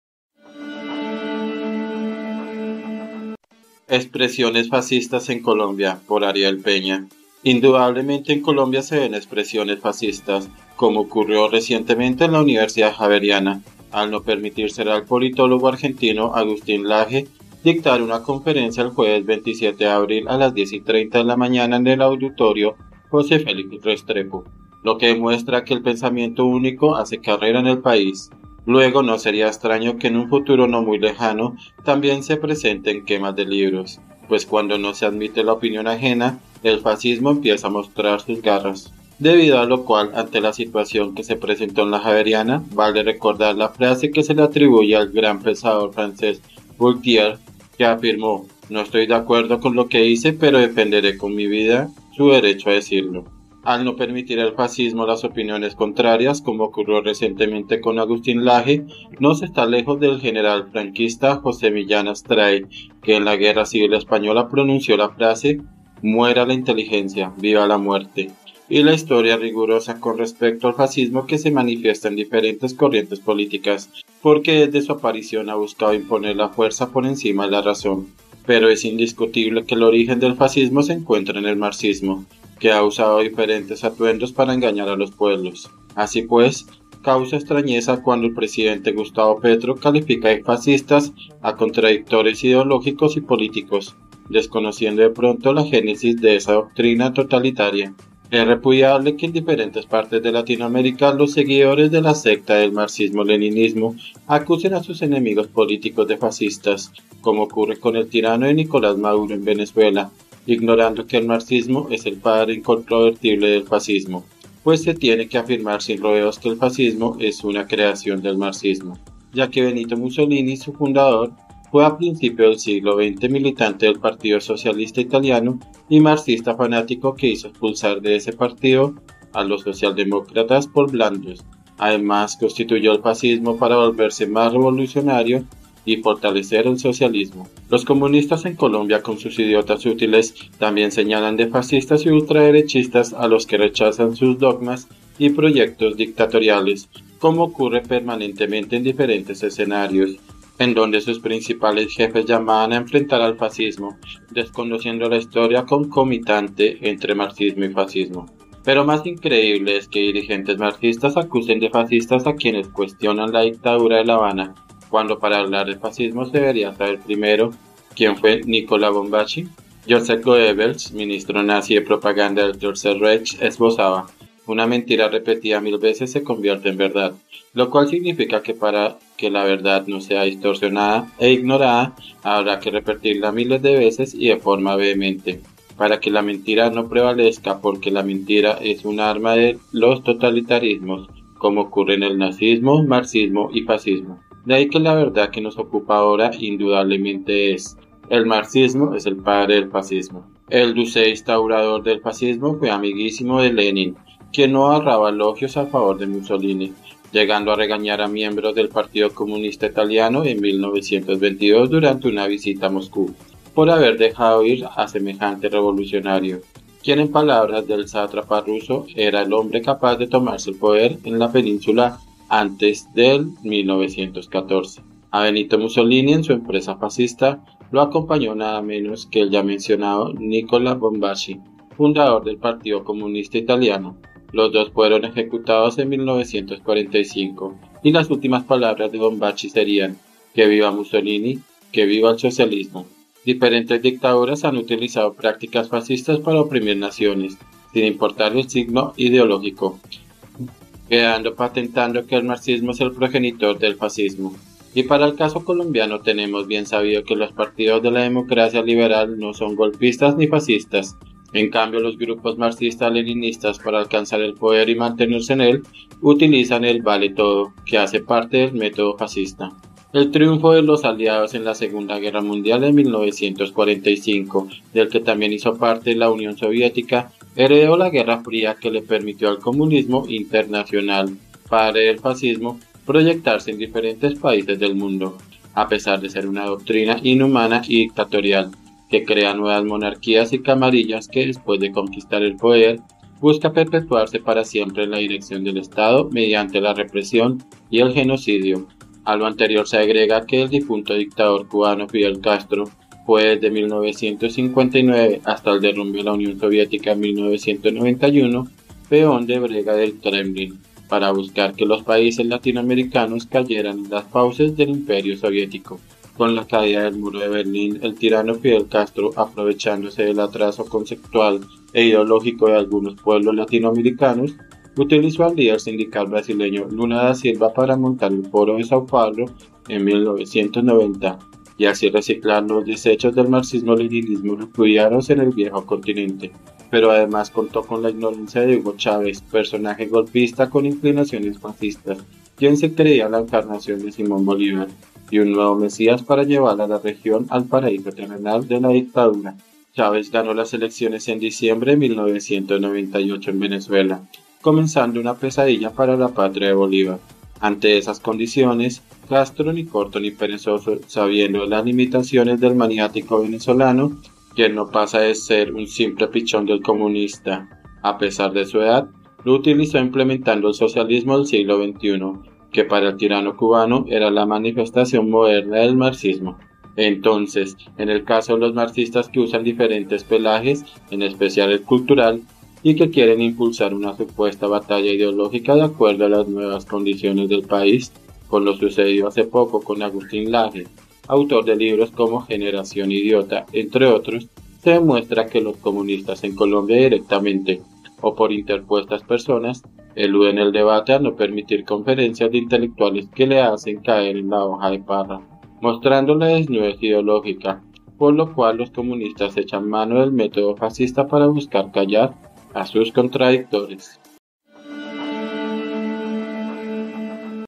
EXPRESIONES FASCISTAS EN COLOMBIA 🎯 Columna Ariel Peña
ndudablemente en Colombia se ven expresiones fascistas, como ocurrió recientemente en la Universidad Javeriana, al no permitírsele al politólogo argentino Agustín Laje, dictar una conferencia el jueves 27 de abril a las 10:30 de la mañana en el auditorio José Félix Restrepo; lo que demuestra que el pensamiento único hace carrera en el país, luego no sería extraño que en un futuro no muy lejano, también se presenten quema de libros, pues cuando no se admite la opinión ajena, el fascismo empieza a mostrar sus garras; debido a lo cual ante la situación que se presentó en la Javeriana, vale recordar, la frase que se le atribuye al gran pensador francés Voltaire, seudónimo de Francois Marie Arouet (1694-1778), que afirmó: “No estoy de acuerdo con lo que dice, pero defenderé con mi vida su derecho a decirlo”.
columna Ariel Peña